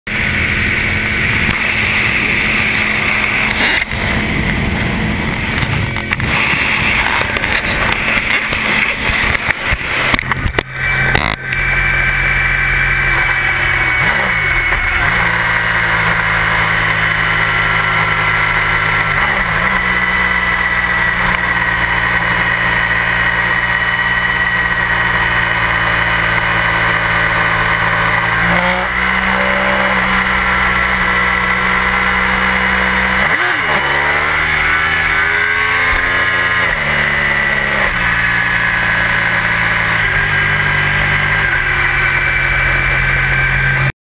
2009 R1 /2006 GSX-R 1000
Playing with the GoPro while ridin